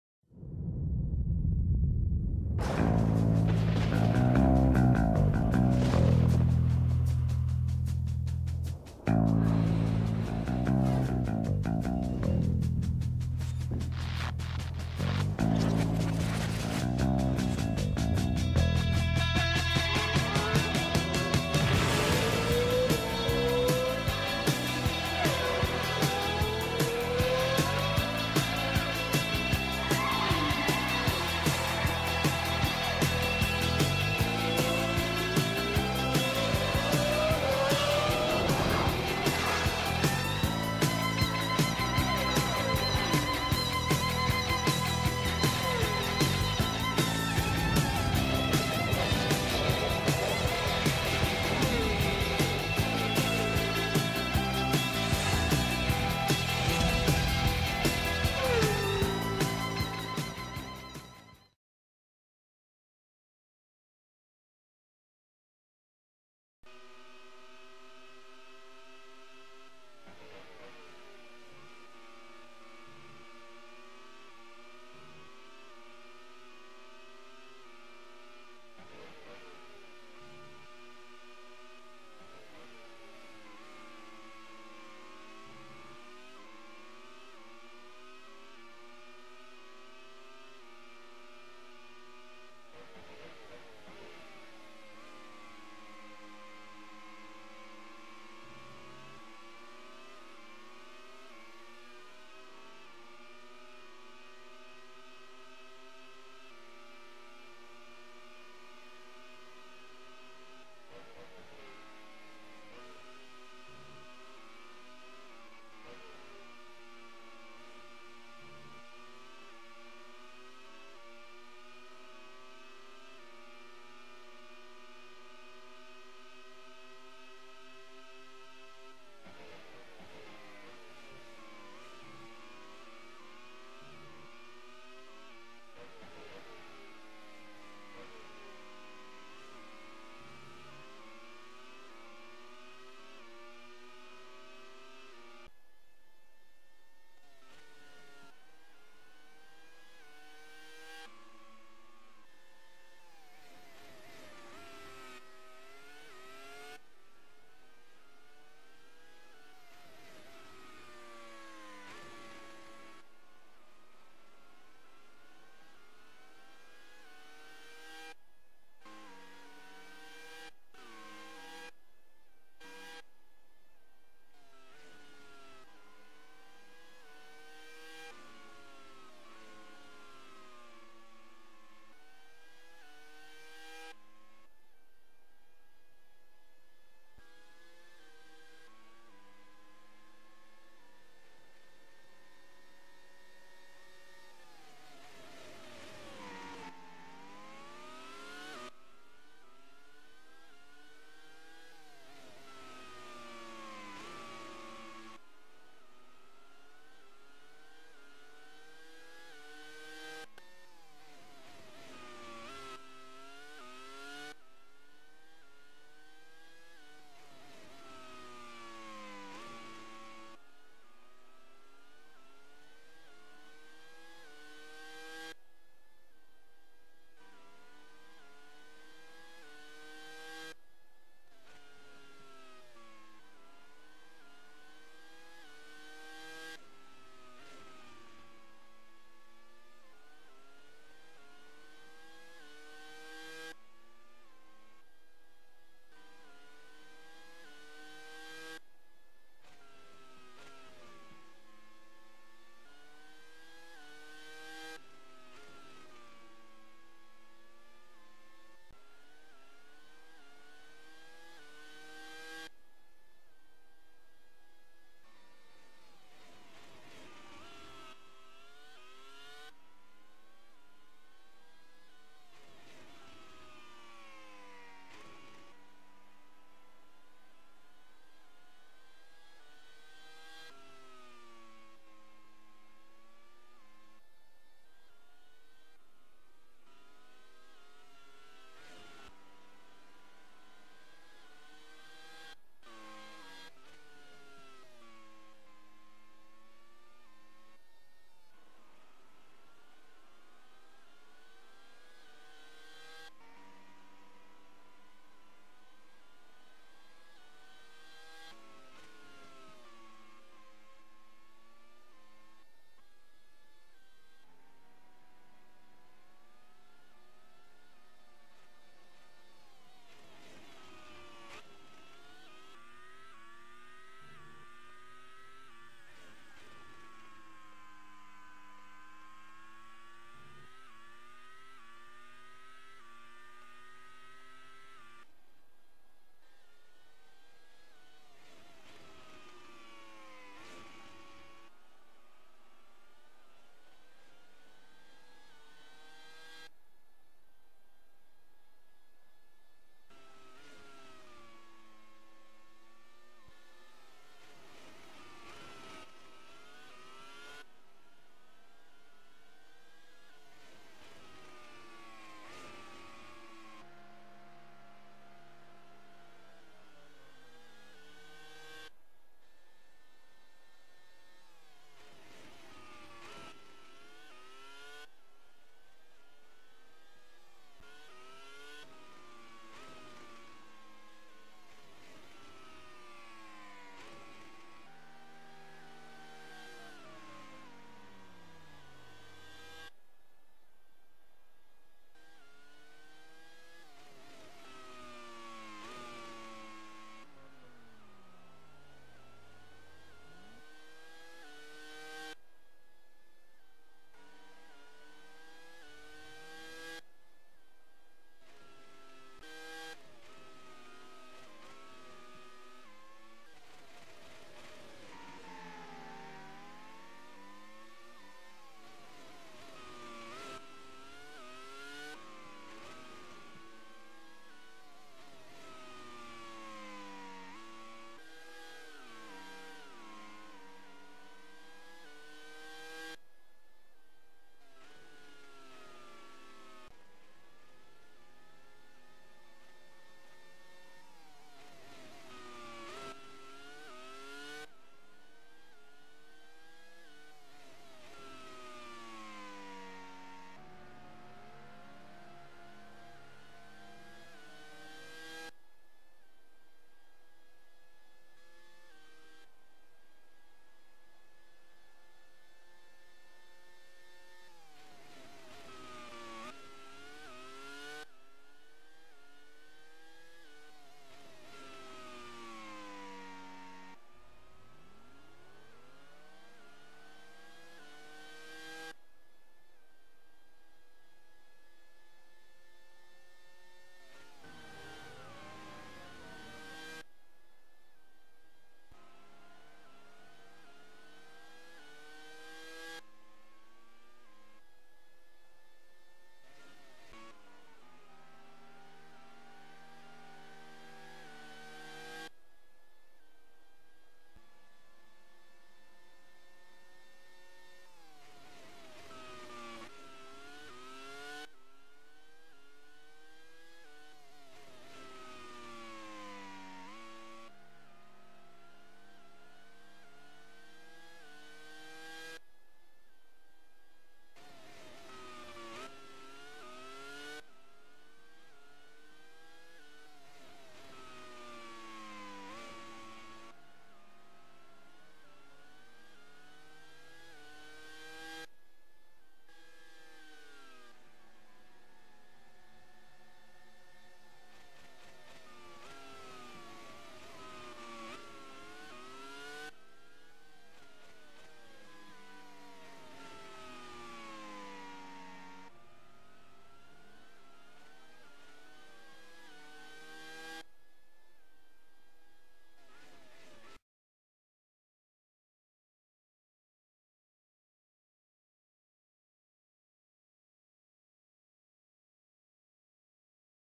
FF1M: 1999 European Grand Prix - Qualifying
Qualifying action from the Nurburgring.